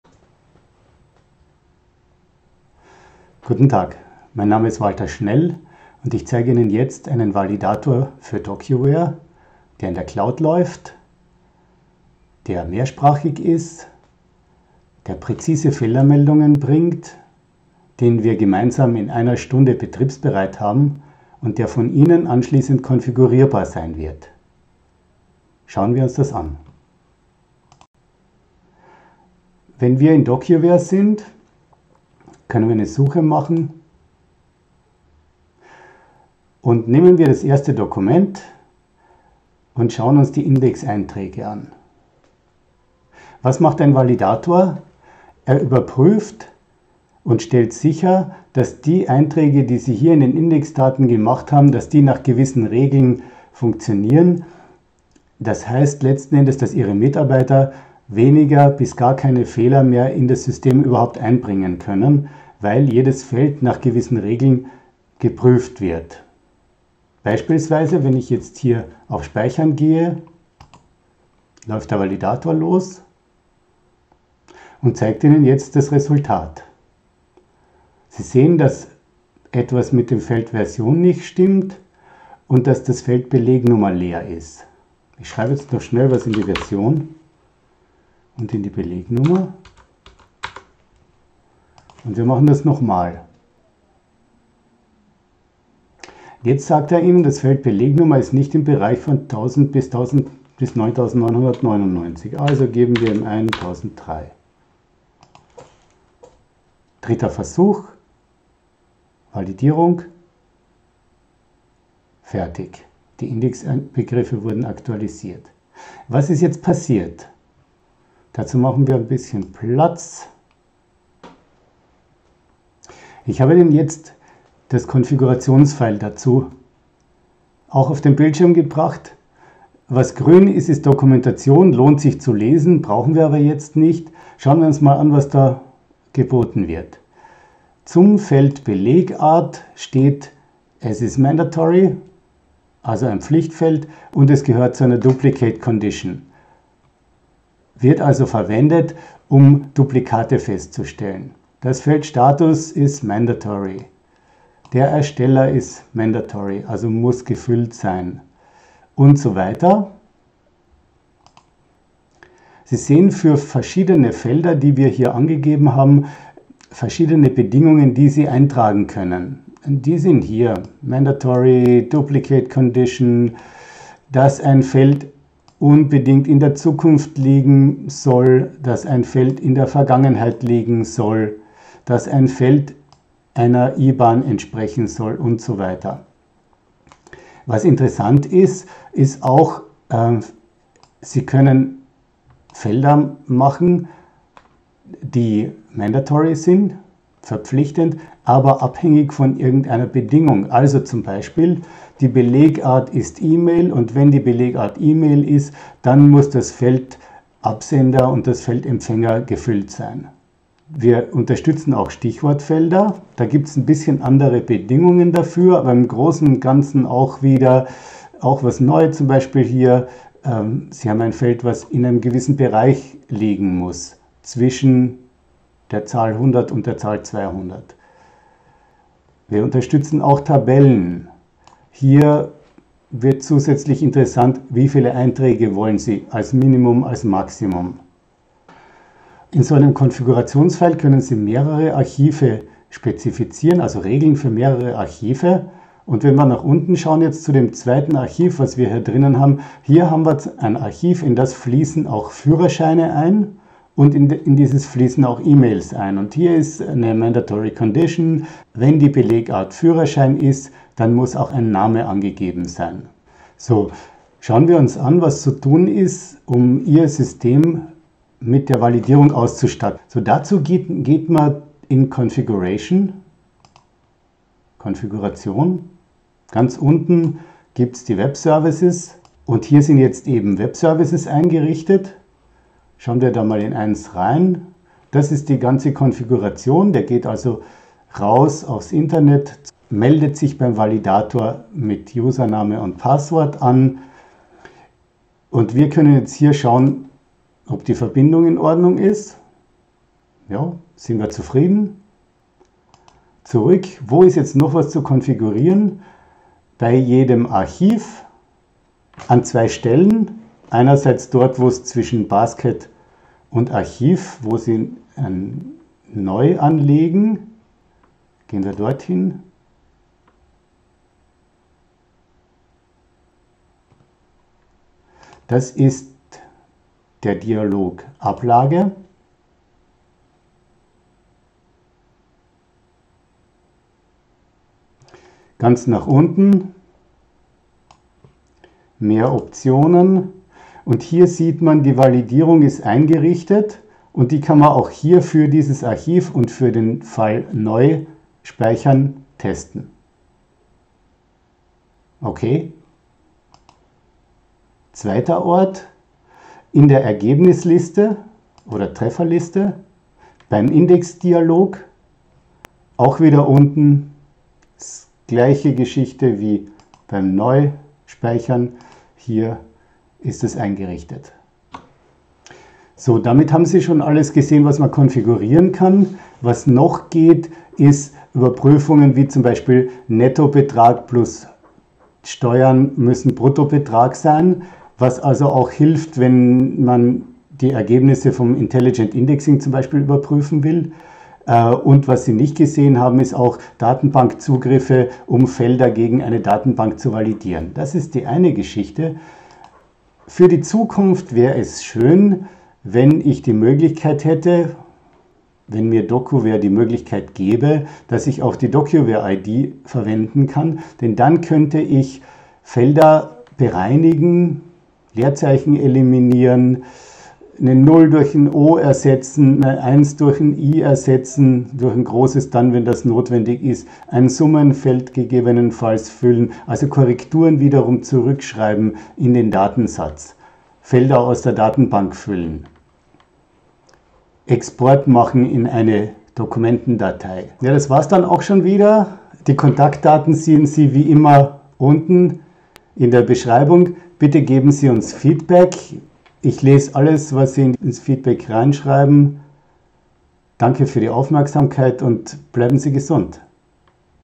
Der Validator [HD Video]
Der Validator überprüft nach konfigurierbaren Regeln die Felder einer Eingabemaske, mehrsprachig, und mit brauchbaren Fehlermeldungen. Stichwortfelder und Tabellen werden dabei unterstützt.

00:30 Beispiel
02:00 Konfiguration von Überprüfungs-Regeln
04:24 Validator einrichten
06:40 weiter gedacht

Roger Goetzendorff
itarius